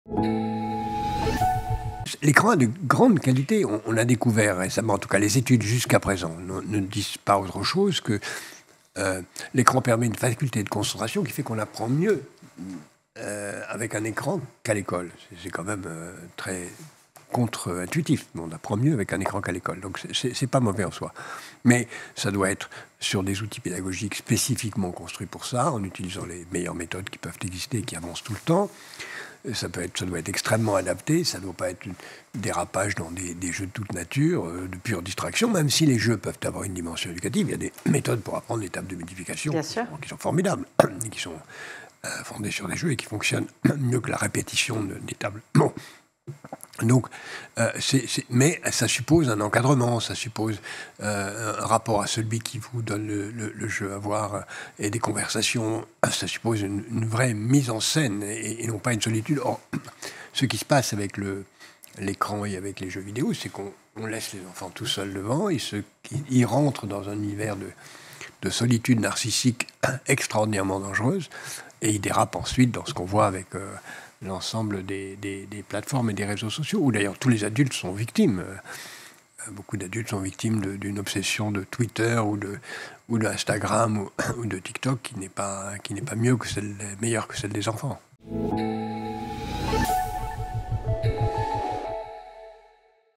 Jacques Attali : "On apprend mieux avec un écran qu'à l'école"
"L'écran est de grande qualité, il permet d'avoir une faculté de concentration. On apprend mieux avec un écran qu'à l'école", souligne Jacques Attali. Il insiste sur la nécessité d'utiliser des outils pédagogiques adaptés.

Haut fonctionnaire, ancien conseiller de François Mitterrand, économiste et écrivain, Jacques Attali vient de publier un nouvel ouvrage intitulé "Histoires et avenirs de l’éducation", un ouvrage dans lequel il expose sa vision de l’école.